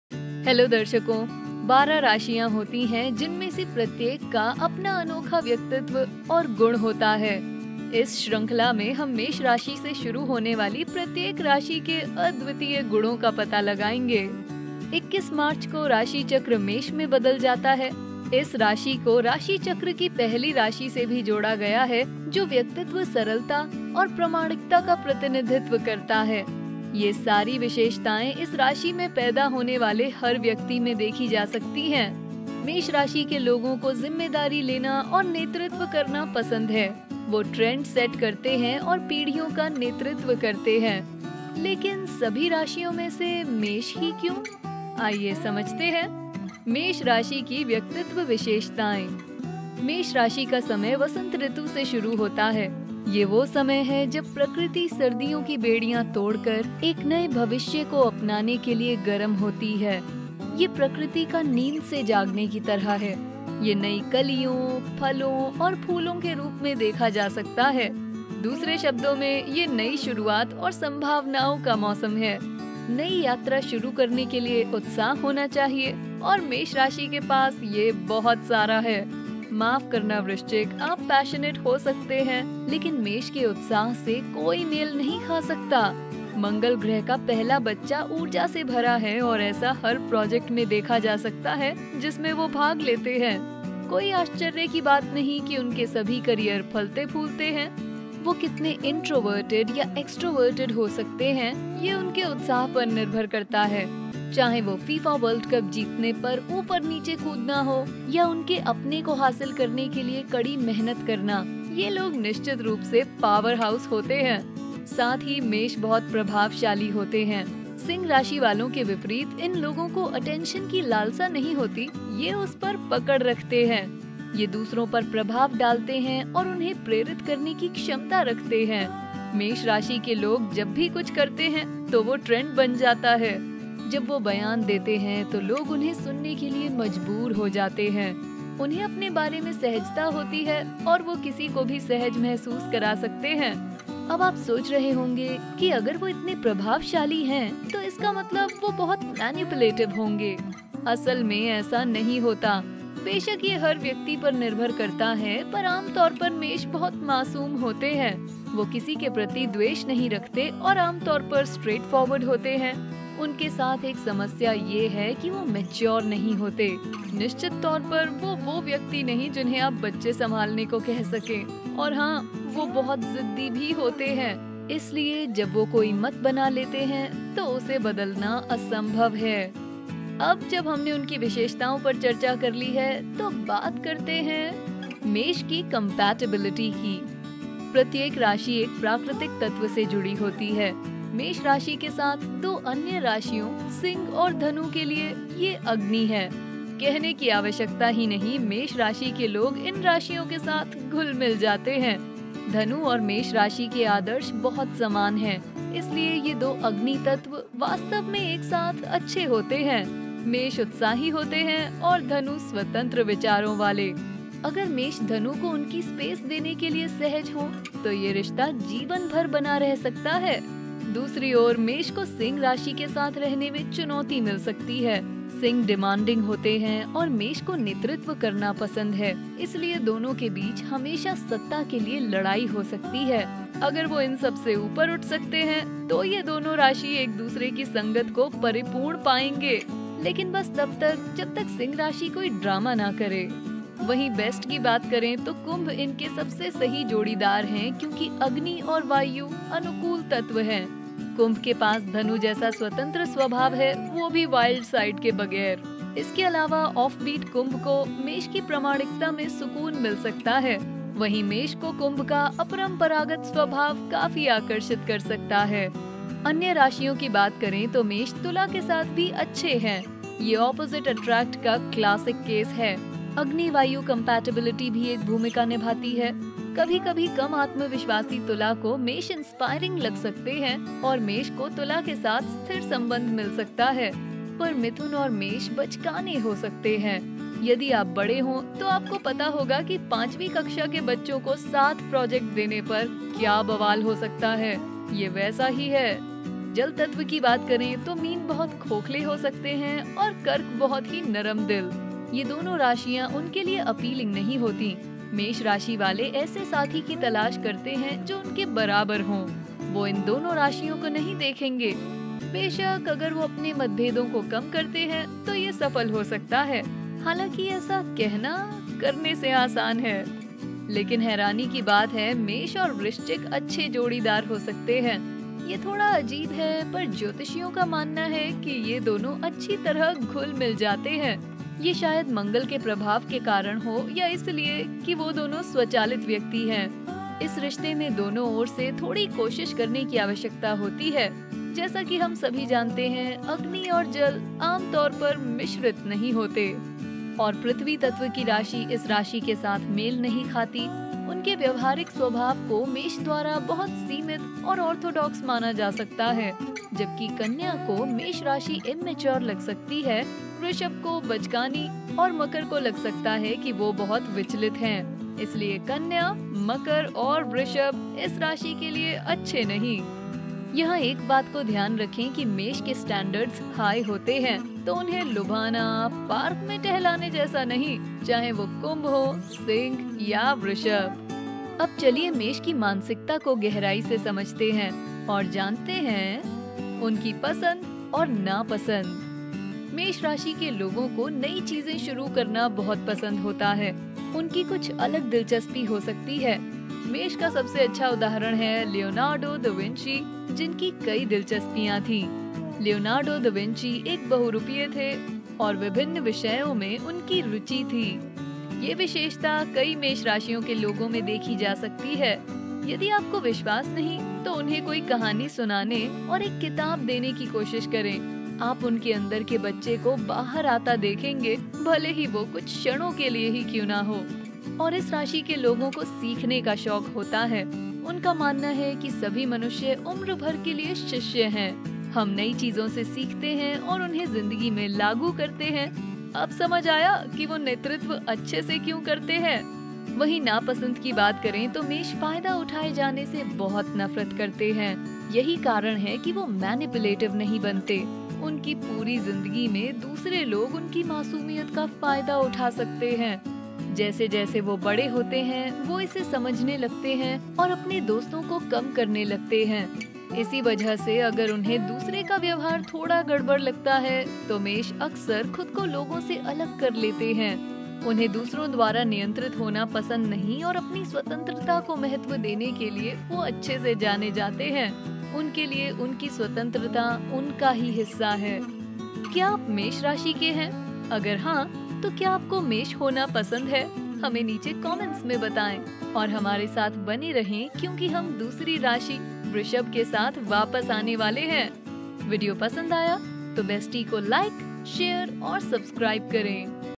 क्या आप मेष राशि के हैं? आपकी अनूठी योग्यताएँ क्या हैं?
मेष की व्यक्तित्व विशेषता:

मेष राशि का समय वसंत ऋतु से शुरू होता है। यह वह समय है, जब प्रकृति सर्दियों की बेड़ियाँ तोड़ कर, एक नए भविष्य को अपनाने के लिए, गरम होती है। यह प्रकृति का नींद से जागने की तरह है। यह नई कलियों, फलों और फूलों के रूप में देखा जा सकता है। दूसरे शब्दों में, यह नई शुरुआत और संभावनाओं का मौसम है। नए यात्रा शुरू करने के लिए, उत्साह होना चाहिए, और मेष राशि के पास यह बहुत सारा है।
माफ करना वृश्चिक, आप passionate हो सकते हैं, लेकिन मेष के उत्साह से, कोई मेल नहीं खा सकता! मंगल गृह का पहला बच्चा ऊर्जा से भरा है, और ऐसा हर project में देखा जा सकता है, जिसमें वे भाग लेते हैं। कोई आश्चर्य की बात नहीं, की उनके सभी career फलते-फूलते हैं। वे कितने introverted है, या extroverted, उस पर उनका उत्साह निर्भर करेगा। चाहे वह FIFA World Cup जीतने पर, ऊपर नीचे कूदना हो, या उनके अपने को हासिल करने के लिए कड़ी मेहनत करना। यह लोग निश्चित रूप से powerhouse है।

पूरा जानने के लिए इस विडियो को अंत तक देखे...